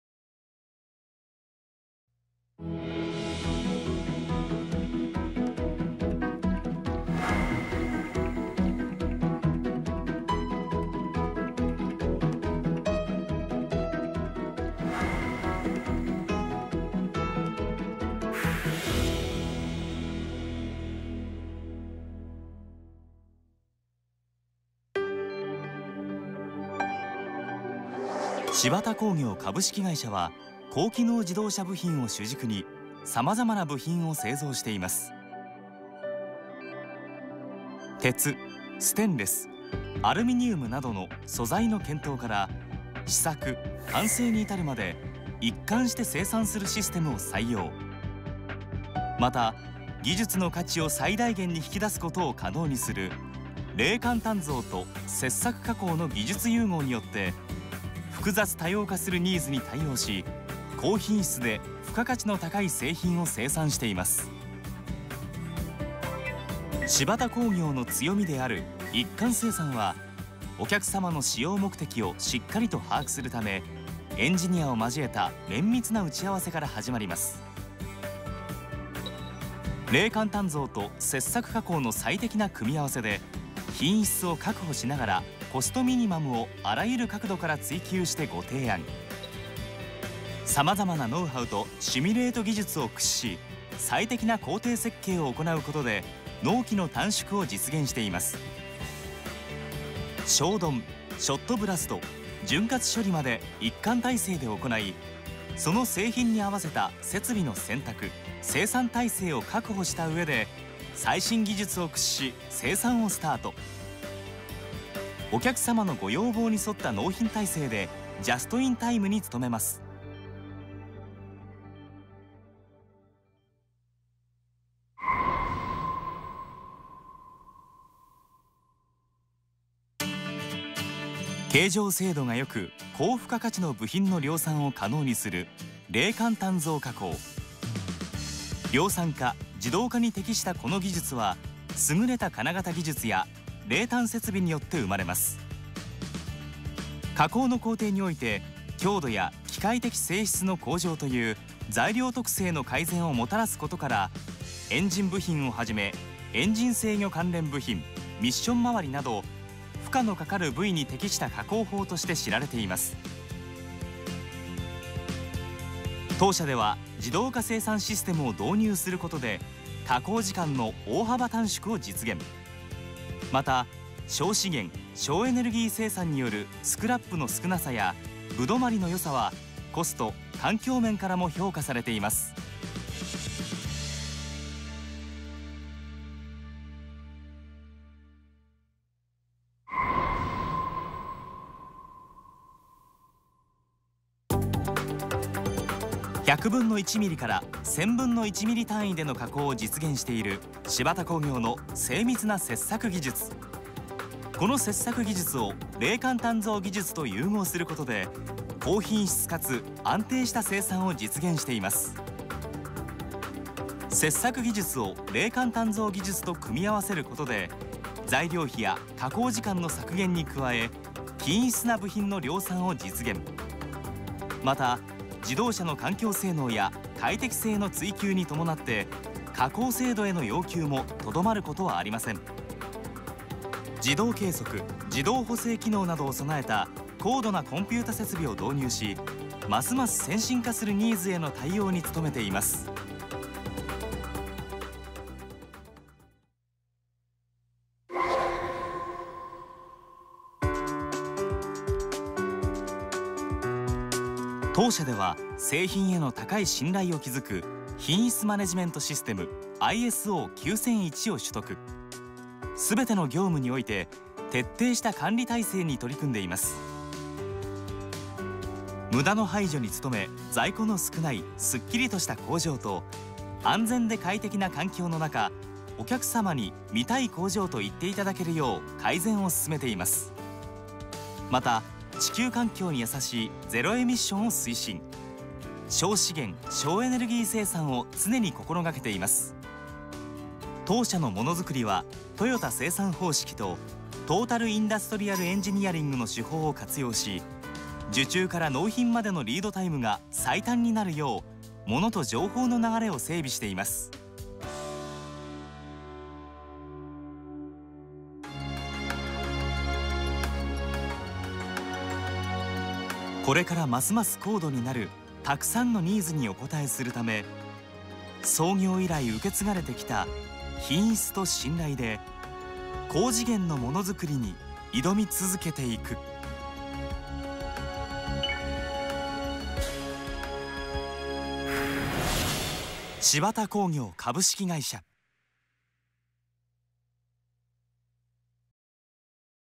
柴田工業株式会社紹介ムービー【柴田工業(株)】
柴田工業の【製品】【魅力】【技術力】がたくさん盛り込まれた会社紹介ムービー

当社は、自動車用部品を中心に様々な高精度、高機能部品を製造しています。最大のメリットは、素材の熱処理、潤滑処理、冷間鍛造、精密切削までの一貫生産体制で製造できることです。設計段階から、冷間鍛造と切削のベストミックスで、エネルギーロスや排出物が少なく、コストミニマムを追求した、最適な加工法をご提案できます。

西尾を動画で盛りアゲる「みてみん西尾」♪